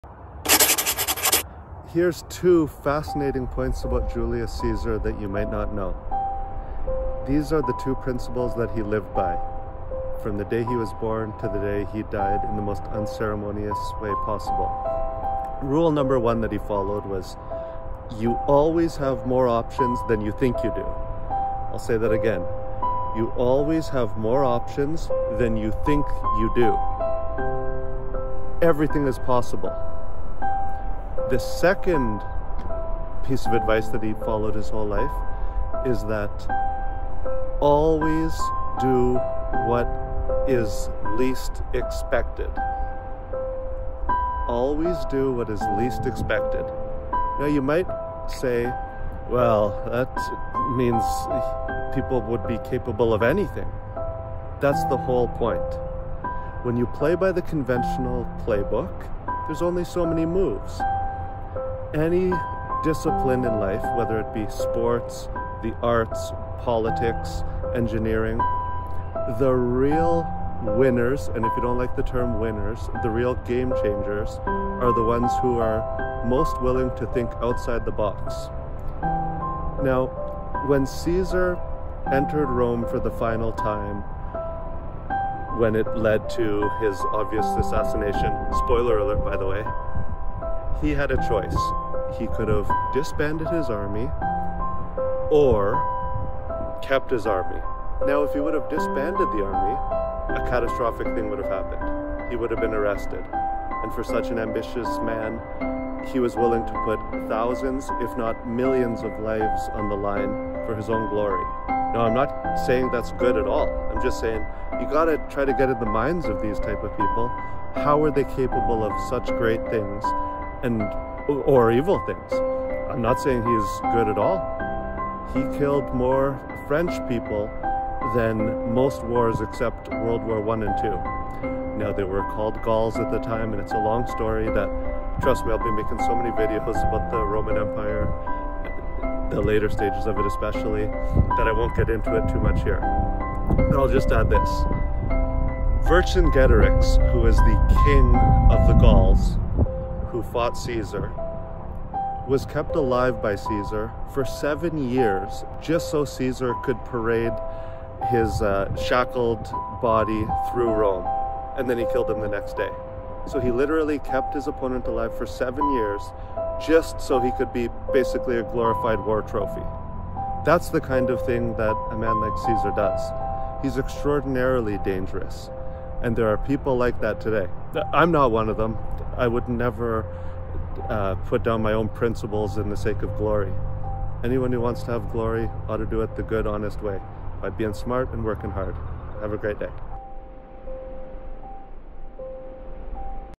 Julius Caesar: Tyrant Or Hero?
Caesar may be the most ambitious and egotistical person in history. This video discusses the pros and cons of his unique mental ability.